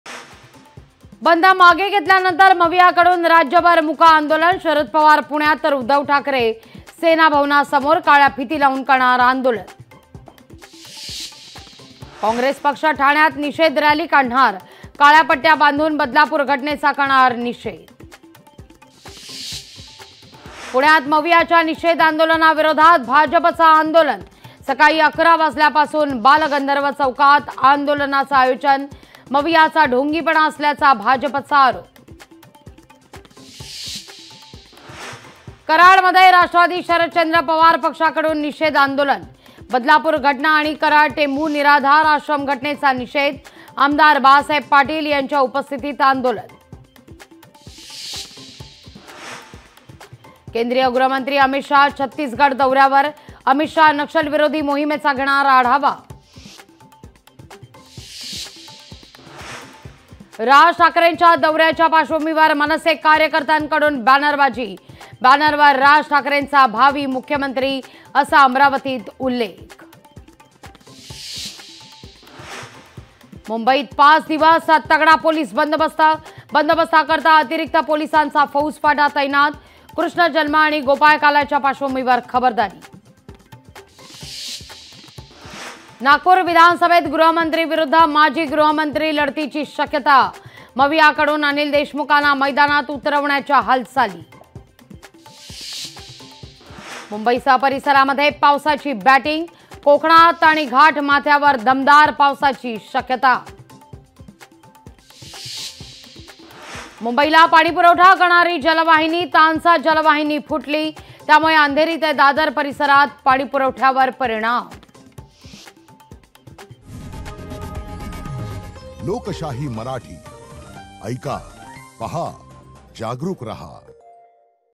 Marathi News Headlines | 9 AM News Today | Maharashtra Politics | Lokshahi Marathi | Aug 24, 2024
Join us as we delve into the heart of Maharashtra Politics, providing comprehensive coverage and unbiased reporting on  platform.